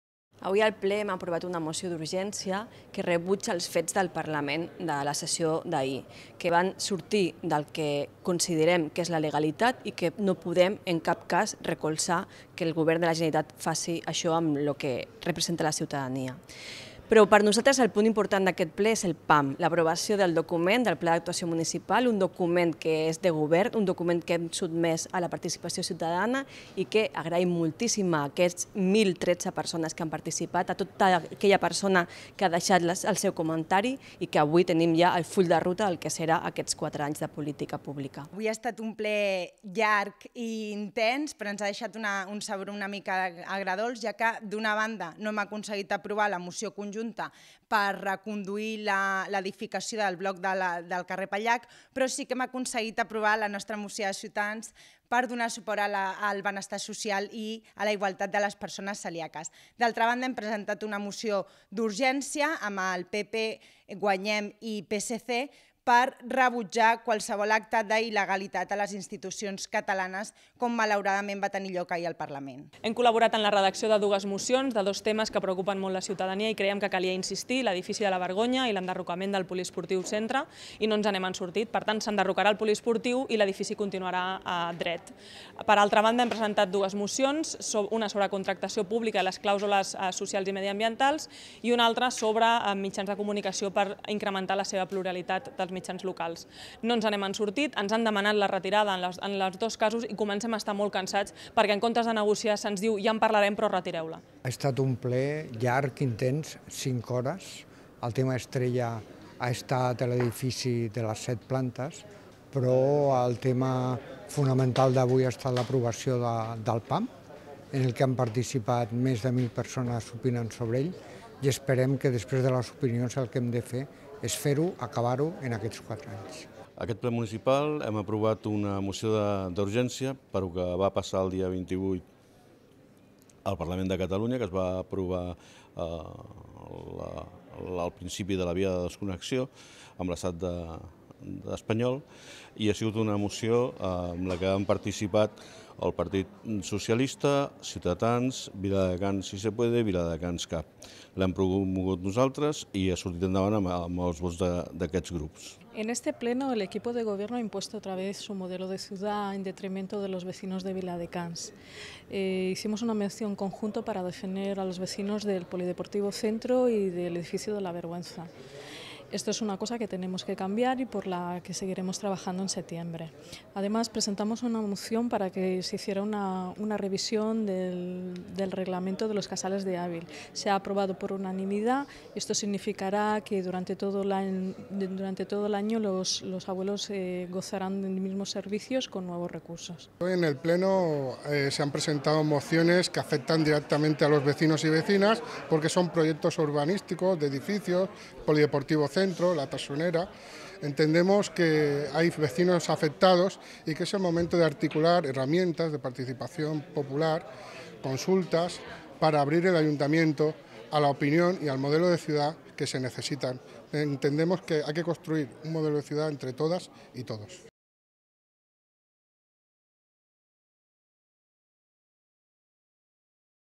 Opinions polítiques del Ple Municipal
Els grups polítics destaquen diferents punts de la sessió de juliol.